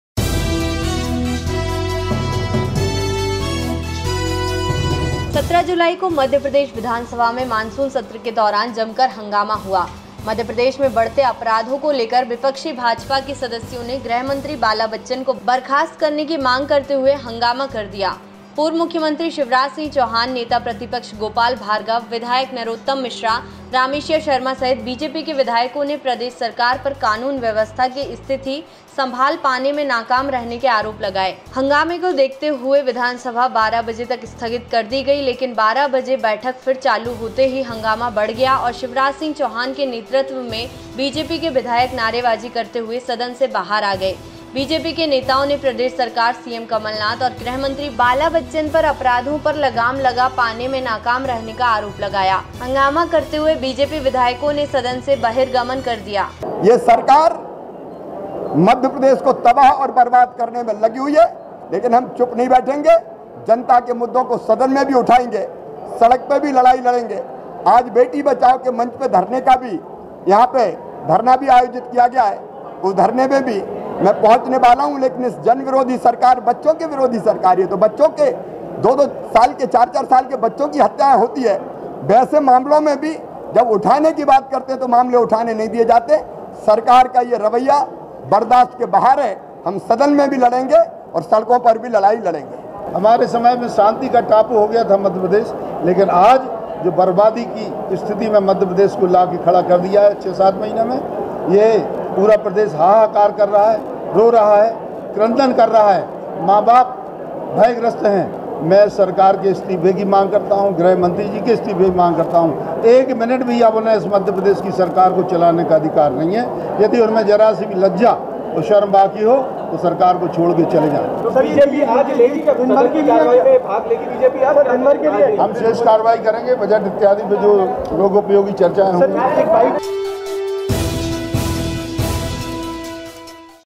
बढ़ते अपराधों को लेकर विधानसभा में जमकर हंगामा, विपक्ष ने किया वॉकआउट
17 जुलाई को मध्यप्रदेश विधानसभा में मानसून सत्र के दौरान जमकर हंगामा हुआ। मध्यप्रदेश में बढ़ते अपराधों को लेकर विपक्षी भाजपा के सदस्यों ने गृहमंत्री बाला बच्चन को बर्खास्त करने की मांग करते हुए हंगामा कर दिया। पूर्व मुख्यमंत्री शिवराज सिंह चौहान, नेता प्रतिपक्ष गोपाल भार्गव, विधायक नरोत्तम मिश्रा, रामेश्वर शर्मा सहित बीजेपी के विधायकों ने प्रदेश सरकार पर कानून व्यवस्था की स्थिति संभाल पाने में नाकाम रहने के आरोप लगाए। हंगामे को देखते हुए विधानसभा 12 बजे तक स्थगित कर दी गई लेकिन 12 बजे बैठक फिर चालू होते ही हंगामा बढ़ गया और शिवराज सिंह चौहान के नेतृत्व में बीजेपी के विधायक नारेबाजी करते हुए सदन से बाहर आ गए। बीजेपी के नेताओं ने प्रदेश सरकार, सीएम कमलनाथ और गृह मंत्री बाला बच्चन पर अपराधों पर लगाम लगा पाने में नाकाम रहने का आरोप लगाया। हंगामा करते हुए बीजेपी विधायकों ने सदन से बहिर्गमन कर दिया।